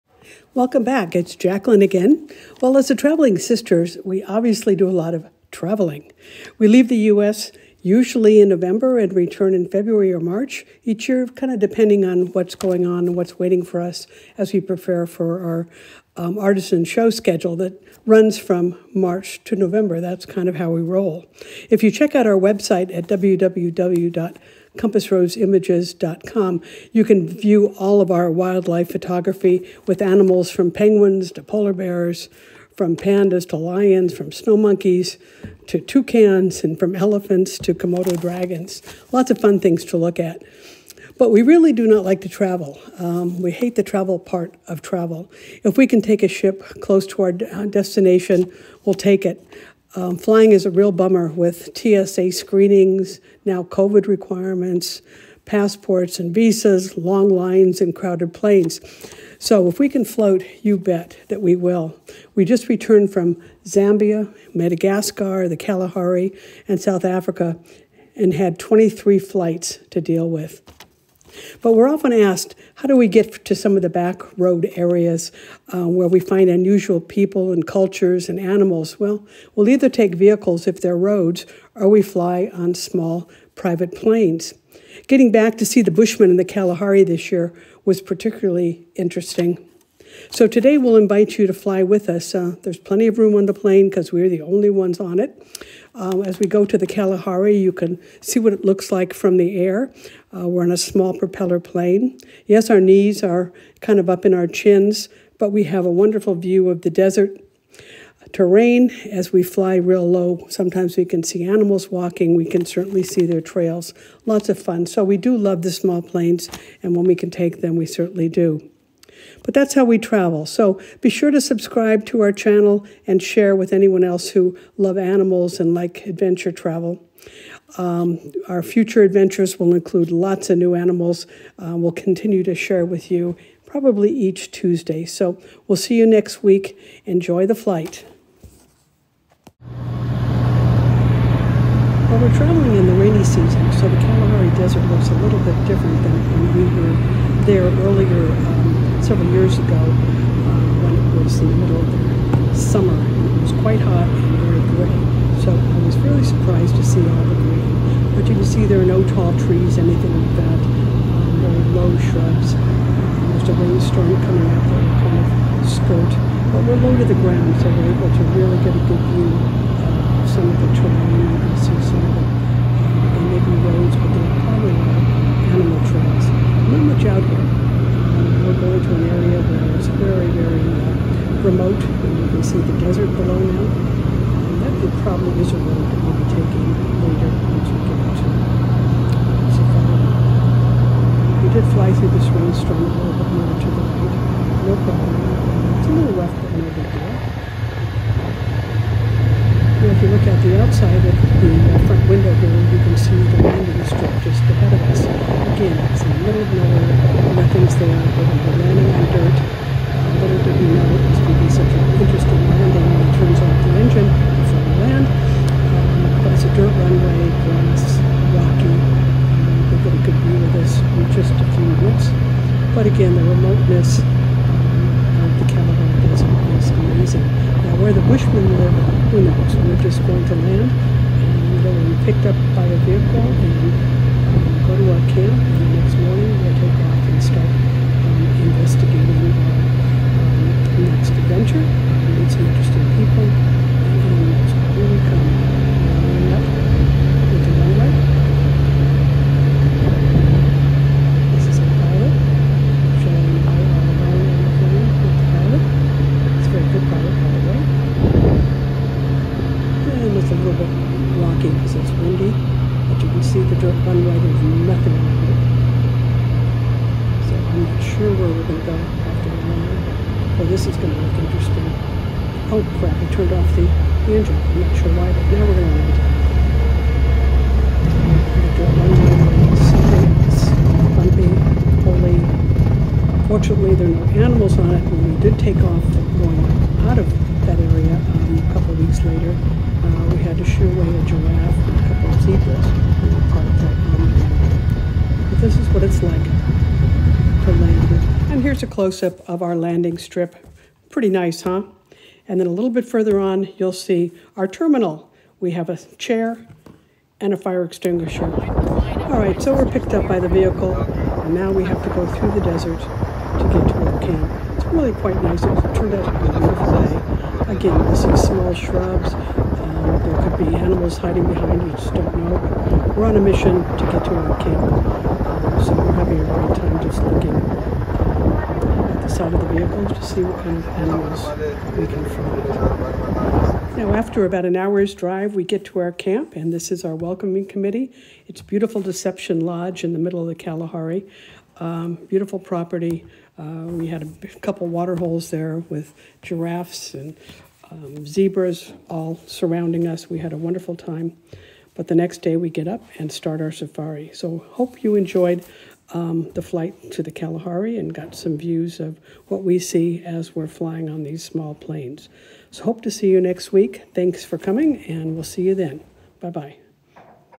How We Get to a Remote Camp in the Kalahari Desert
Hop aboard a small plane with the traveling sisters as they share aerial views of the expansive Kalahari Desert on their way to a remote camp to meet the Bushmen who call this unforgiving environment home.